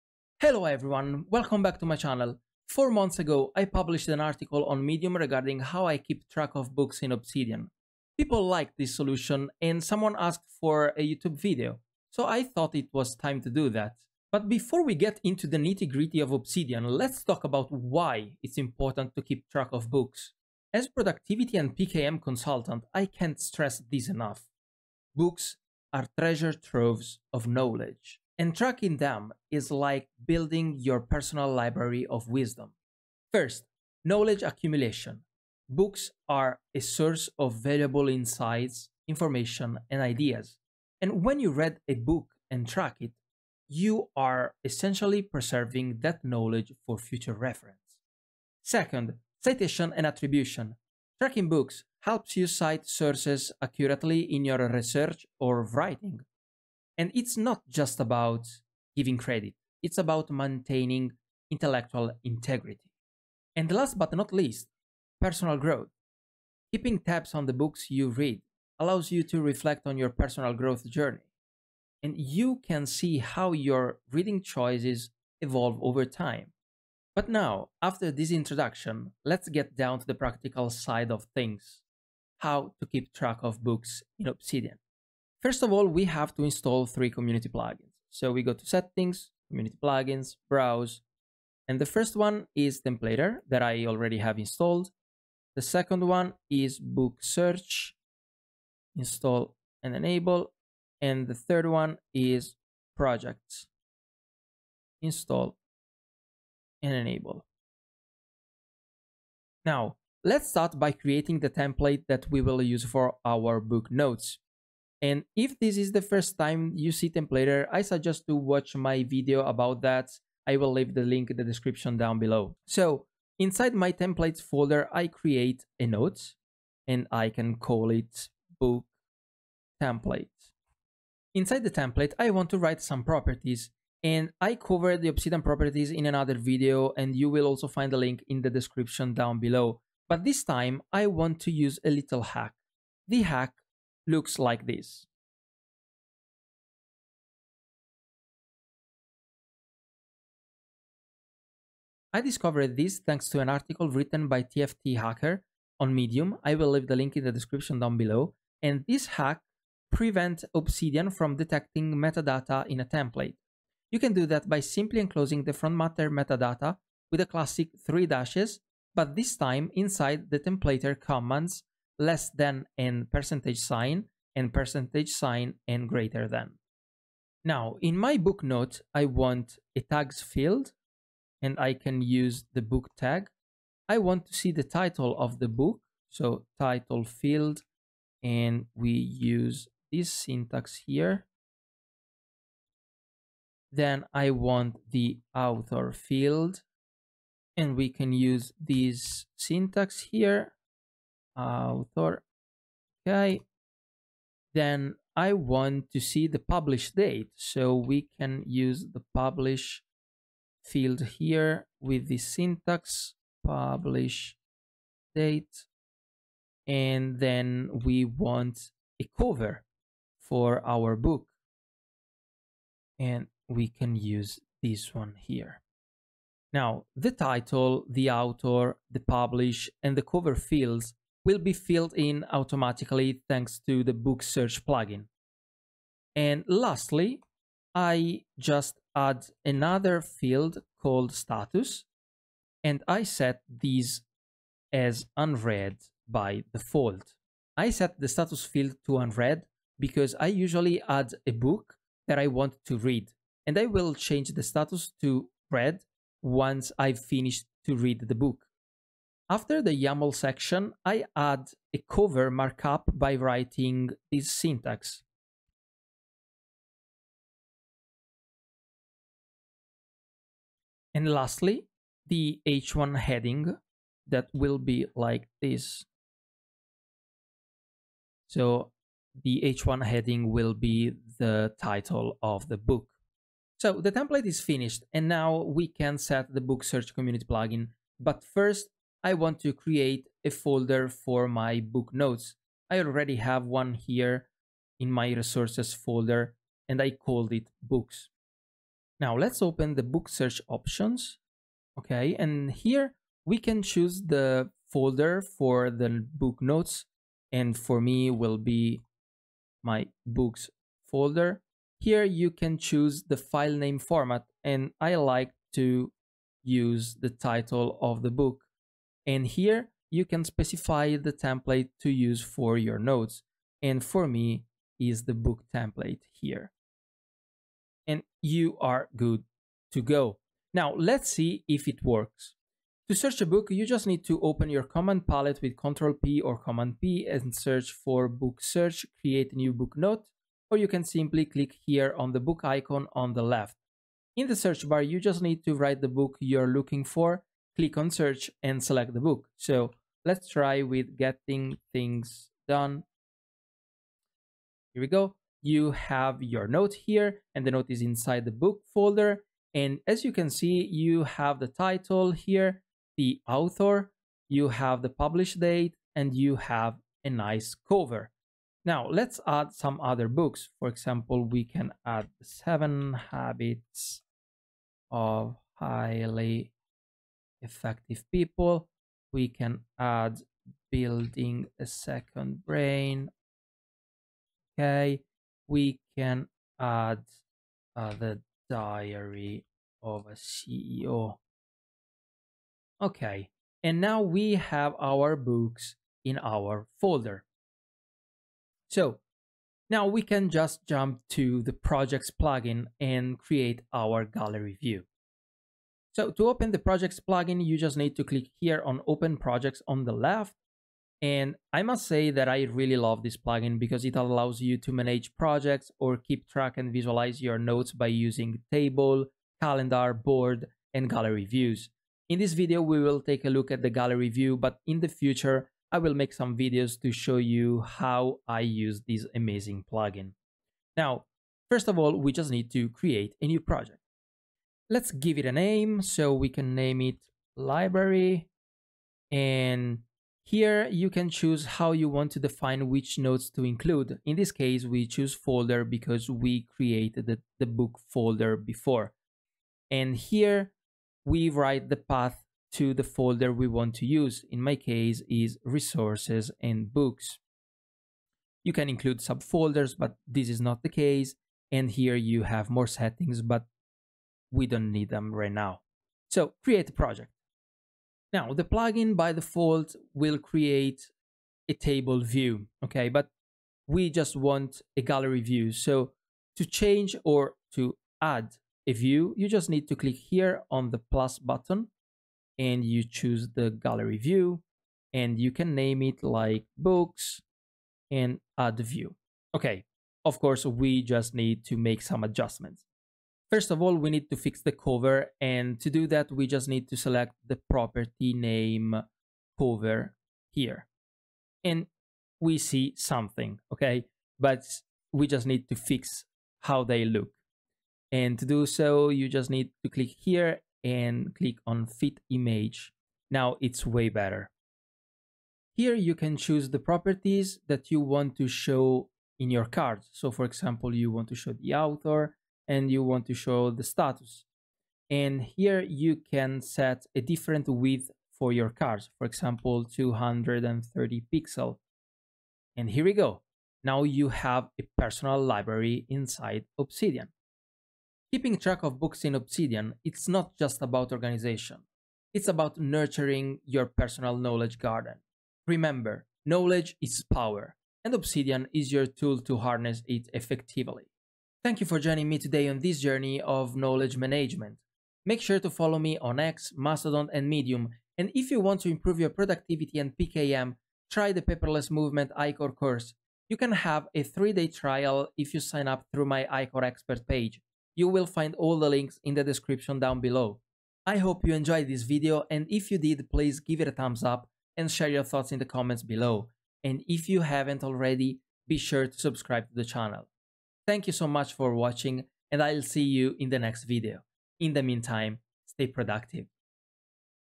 Obsidian Library: How To Keep Track of Your Books in Obsidian
Today I want to show you how you can keep track of books in your Obsidian Vault.
Books are treasure troves of knowledge, and tracking them is like building your personal library of wisdom.

📝 This is the Book Template structure if you want to copy/paste it

-- TIMESTAMPS --
00:00 Introduction
00:20 Keep tracks of books
01:25 Community Plugins
01:50 The Template
04:45 Book Search Community Plugin
05:32 How to Create Books Notes
06:45 Projects Community Plugin
08:58 Last Thoughts